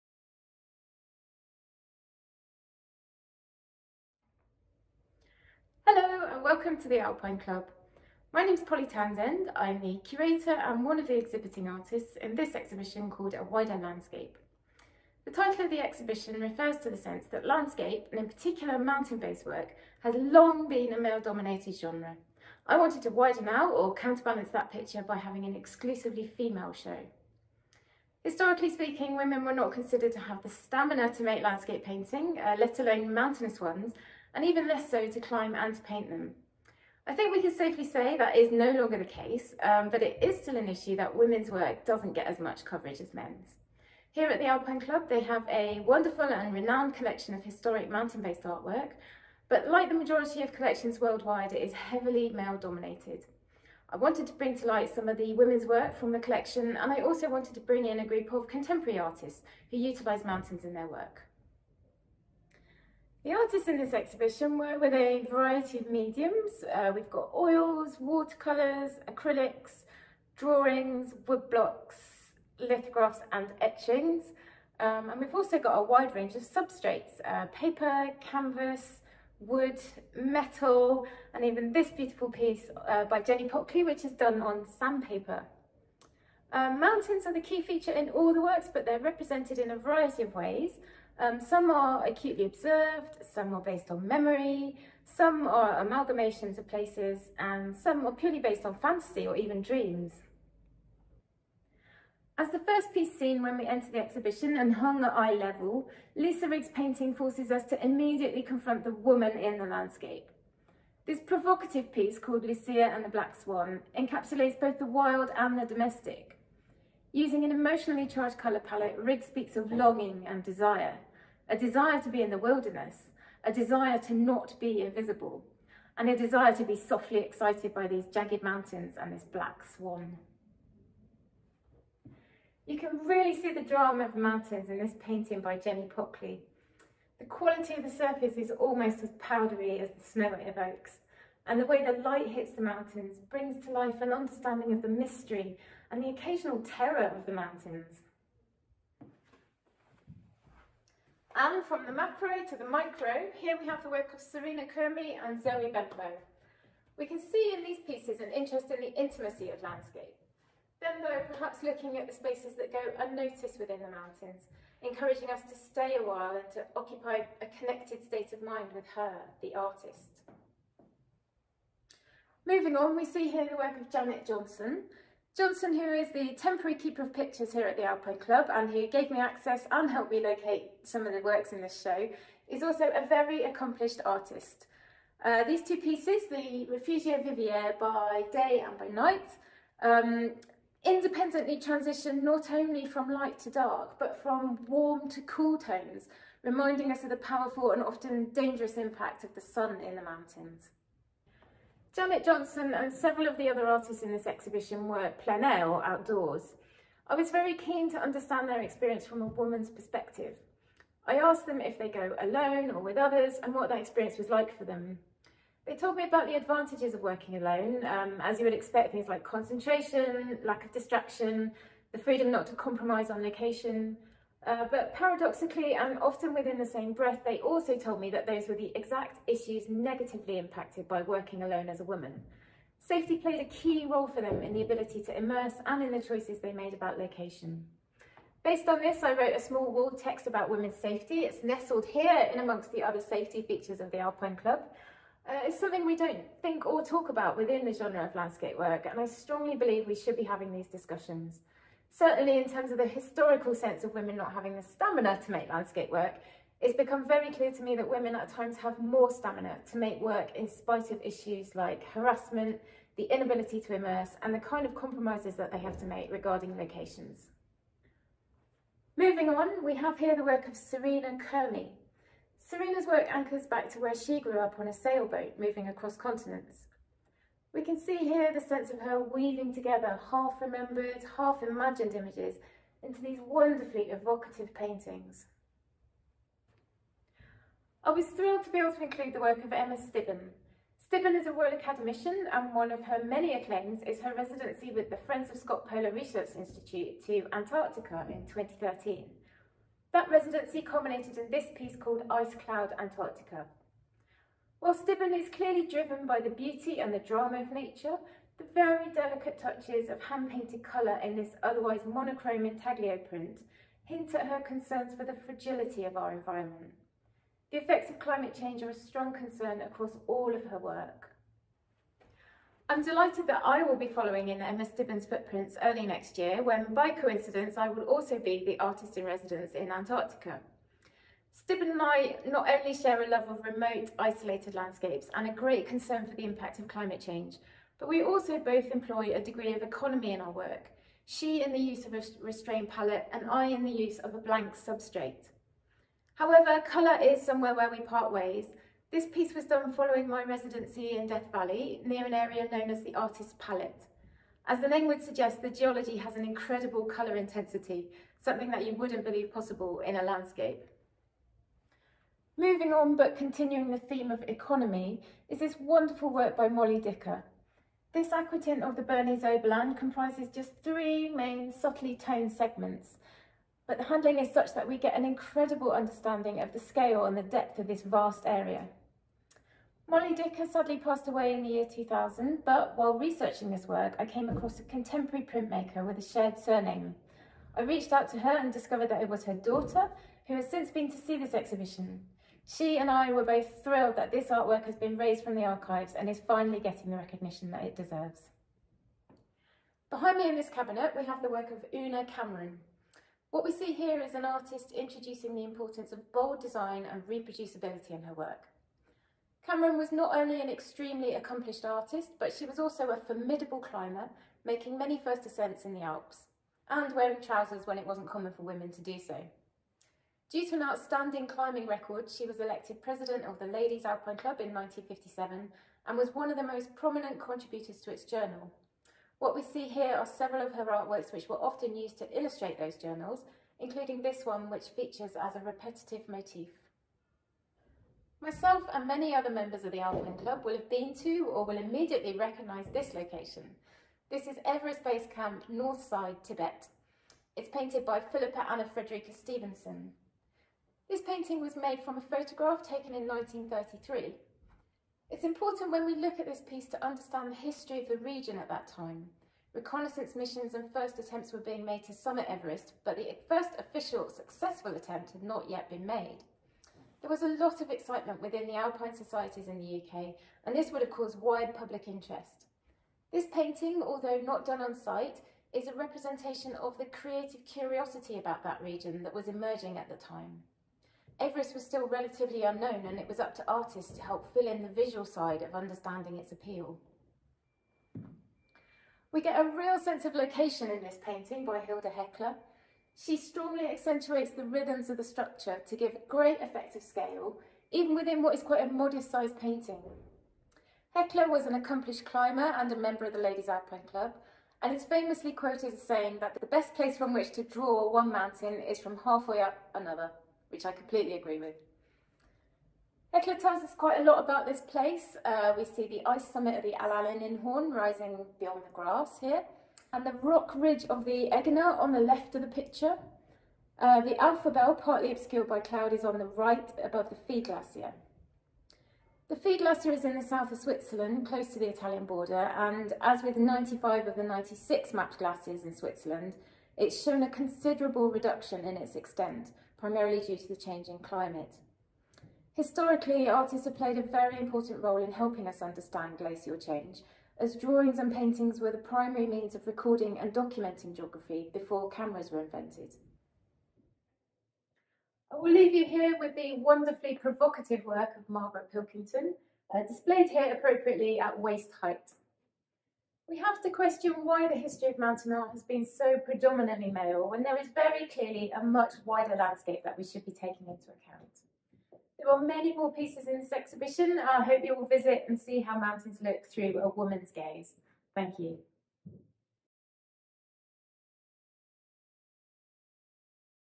A Wider Landscape - Video Tour
'A Wider Landscape', an exhibition of landscape art past and present by female artists ran at the Alpine Club's London exhibition space from 13 September 2022 to 19 November 2022. In this video, the exhibition's curator Polly Townsend gives us a guided tour of some of the works on display, discussing the mediums used by the various artists, explaining what their work represents and offering some history on the older pieces.

You can learn more about this exhibition via the Alpine Club

This video was shot, edited and presented by Polly Townsend.